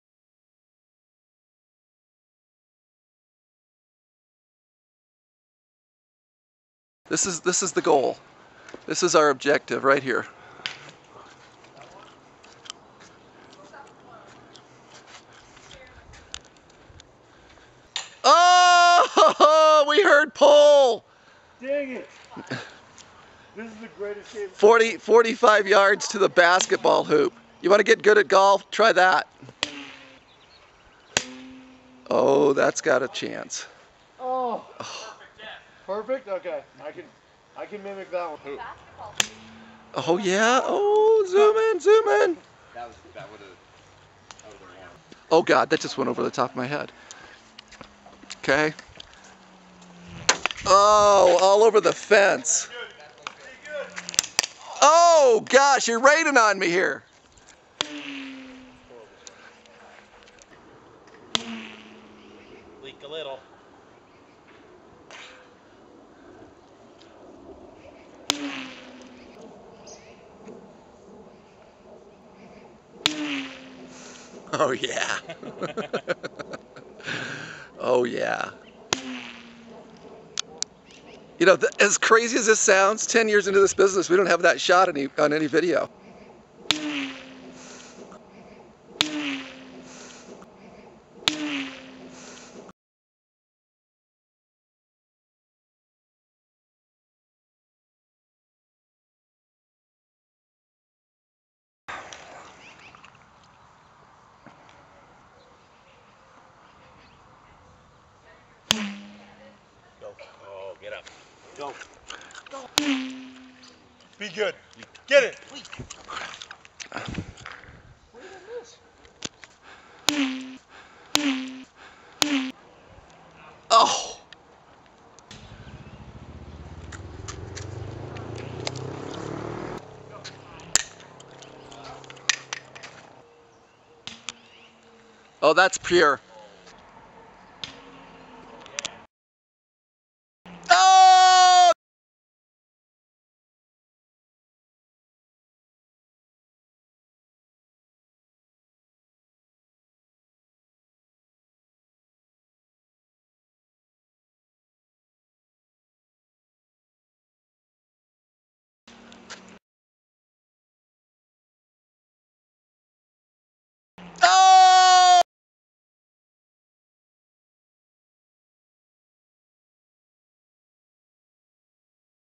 BasketBirdieBall How good are you! BirdieBall into a BasketBall Hoop? Try it and win!
BasketBirdieBall How good are you! Send video of a make from 40 Yards, win a green! Make this shot, I don't care how many takes, it requires, receive a free putting green. This offer can be pulled at any time but I doubt I am going to have too!